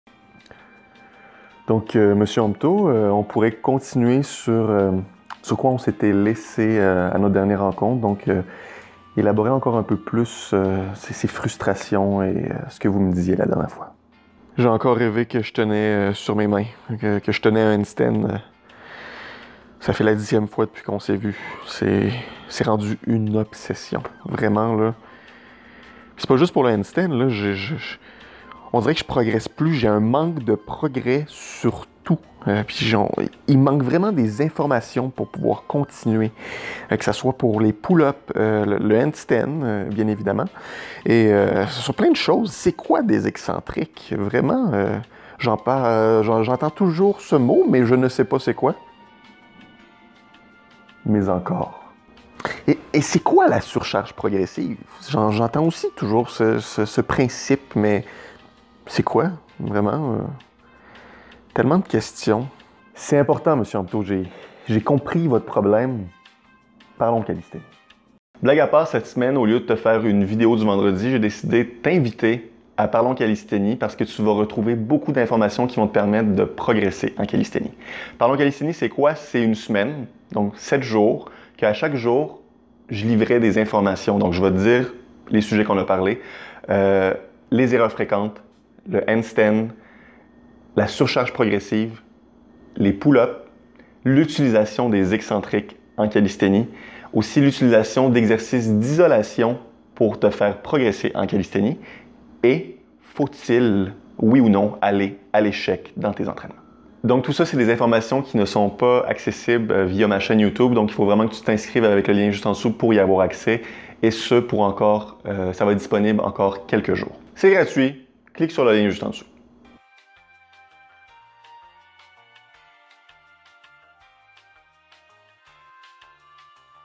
Parlons callisthénie chez le psy
Cette semaine, plutôt que de vous présenter une vidéo du vendredi tutoriel comme à l'habitude, j'ai pensé vous inviter à participer à Parlons callisthénie, un événement de 7 jours gratuit où je partage des informations sur la callisthénie (que je ne partage pas sur YouTube ni nulle part ailleurs). Parlons callisthénie tire à sa fin, mais le contenu est encore disponible quelques jours, alors je vous invite à vous inscrire rapidement. Voici les thématiques de chacune des journées :
• Jour 1 : Les erreurs fréquentes
• Jour 2 : L'équilibre sur les mains
• Jour 3 : La surcharge progressive
• Jour 4 : Les pull-ups
• Jour 5 : Les excentriques
• Jour 6 : Les exercices d'isolation
• Jour 7 : Aller à l'échec

Bon vendredi!